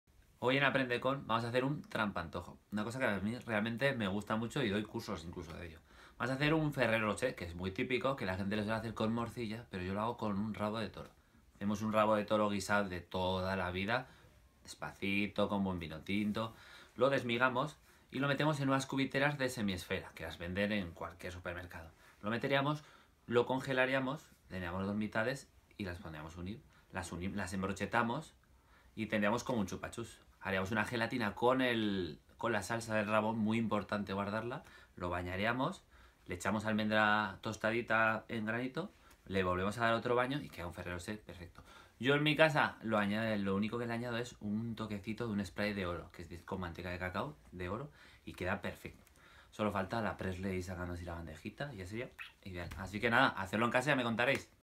Aprende con Toño Rodríguez
Toño Rodríguez nos cuenta el secreto de los trampantojos y nos enseña a hacer un Ferrero Rocher con rabo de toro.